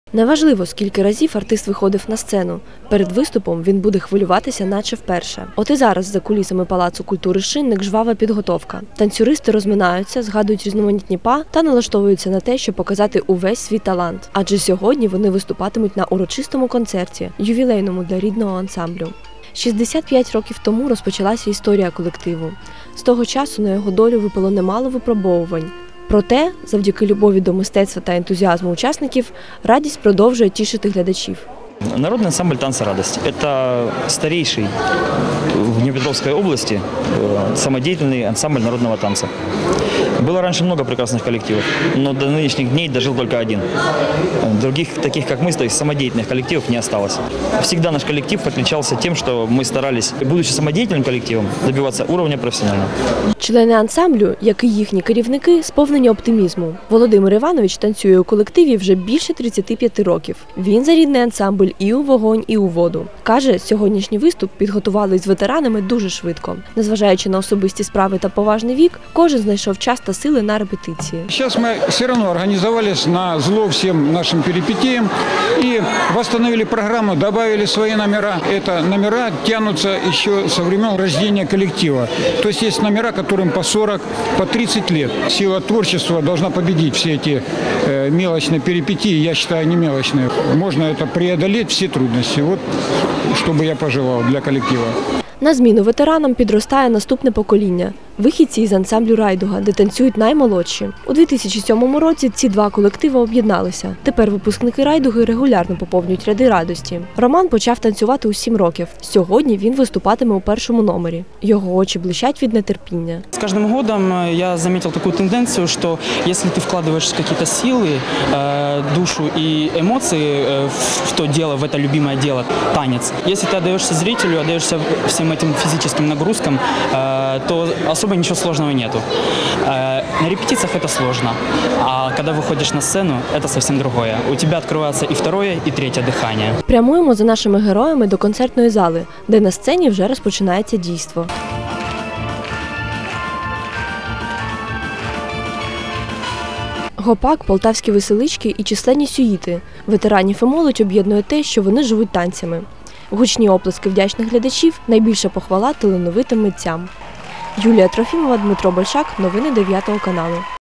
Первые участники ансамбля "Радость" снова вышли на сцену
Старейшемй любительскому ансамблю Днепропетровска исполнилось 65 лет